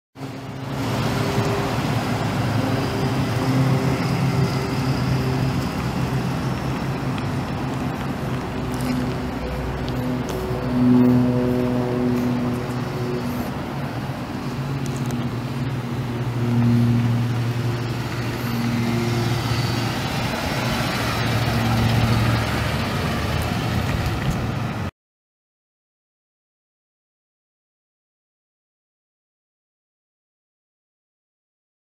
2010 Ford F150 SuperCab pickup truck for sale at auction | bidding closes June 19, 2018
2010 Ford F150 SuperCab pickup truck
101,823 miles on odometer
5.4L V8 SOHC 24V FFV gas engine
Automatic transmission
Tilt
Cruise
AM/FM
Power windows, locks, and mirrors
Vinyl interior
Front tow hooks
Receiver hitch
Trailer wiring
Goodyear 265/70R17 tires
Oklahoma title